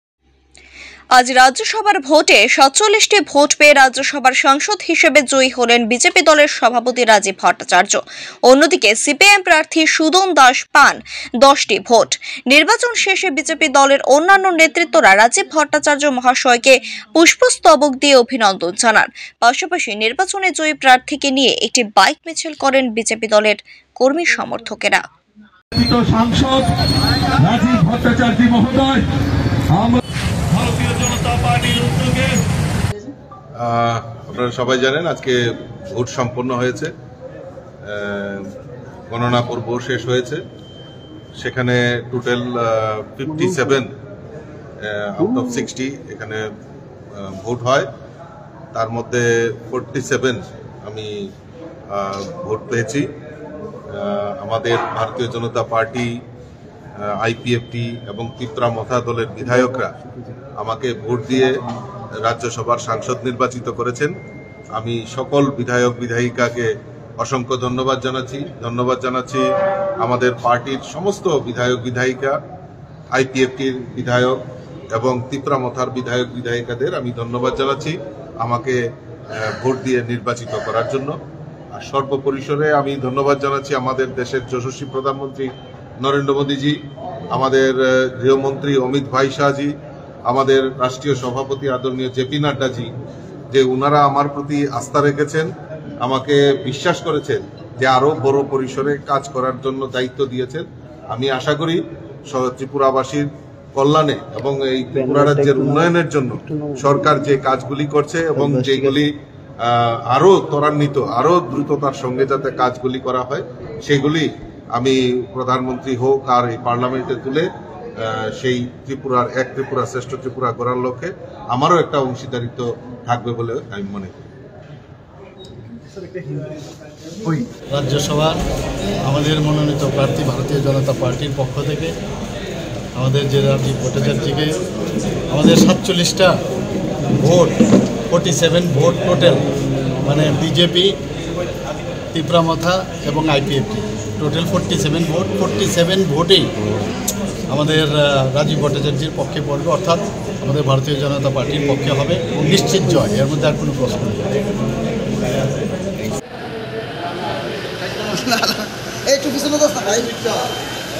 ত্রিপুরা রাজ্য সভা ভোটে জয়ী হলেন বিজেপি দলের প্রাথী রাজীব ভট্টাচার্জি | Rajib Bhattacharjee
News Details :-
আজ রাজ্য সভার ভোটে 47 টি ভোট পেয়ে রাজ্যসভার সাংসদ হিসেবে জয়ী হলেন বিজেপি দলের সভাপতি রাজিব ভট্টাচার্য। অন্যদিকে সিপিআইএম প্রার্থী সুদন দাস পান  ১০ টি ভোট । নির্বাচন শেষে বিজিপি দলের অন্যান্য নেতৃত্বরা রাজীব ভট্টাচার্য্য মহাশয়কে পুস্প স্তবক দিয়ে অভিনন্দন জানান। পাশাপাশি নির্বাচনে জয়ী প্রার্থীকে নিয়ে একটি বাইক মিছিল করলেন বিজেপি দলের কর্মী সমর্থকেরা।